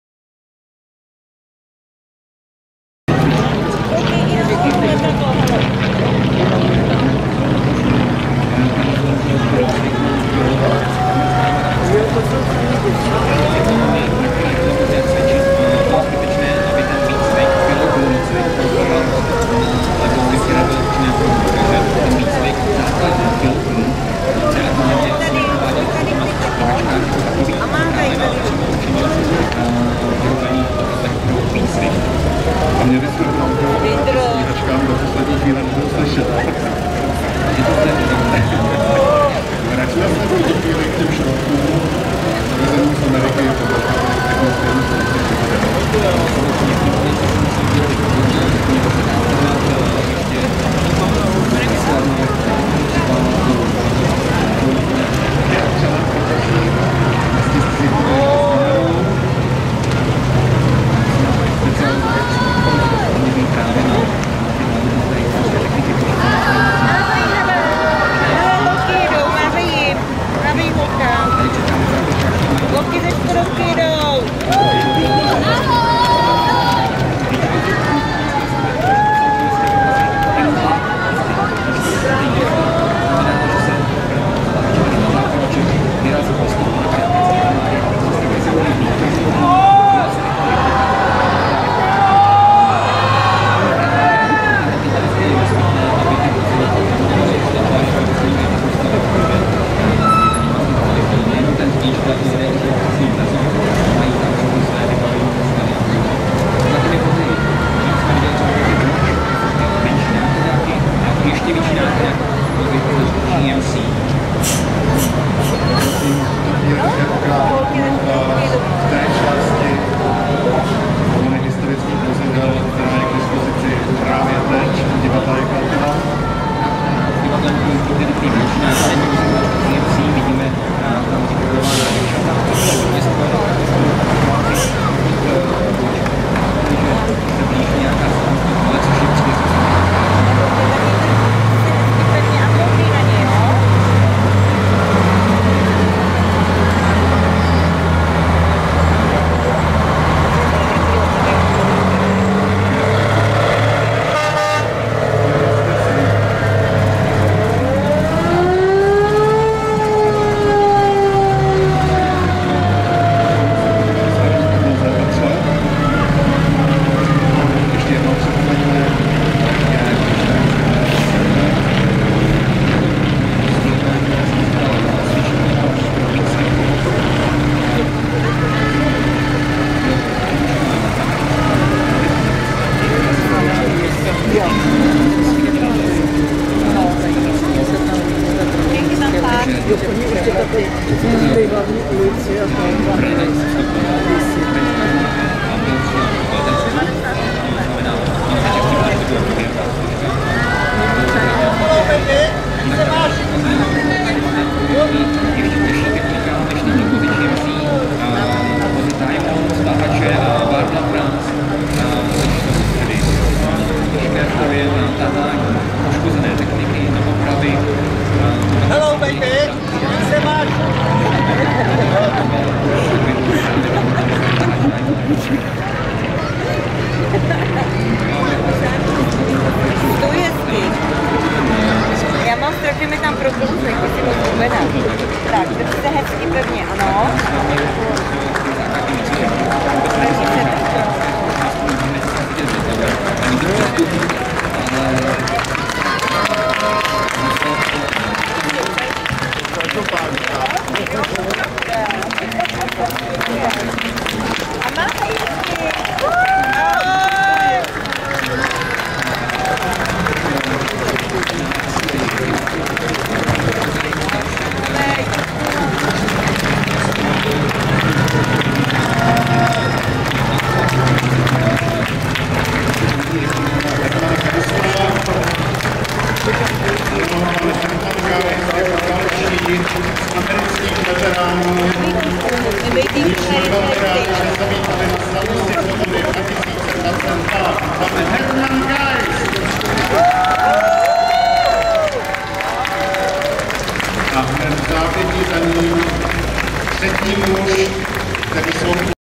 Konvoj svobody - 2 díl,Plzeň 8. 5. 2022
Aniversario 77 de la liberacion de Plzeň por el ejercito de los Estados Unidos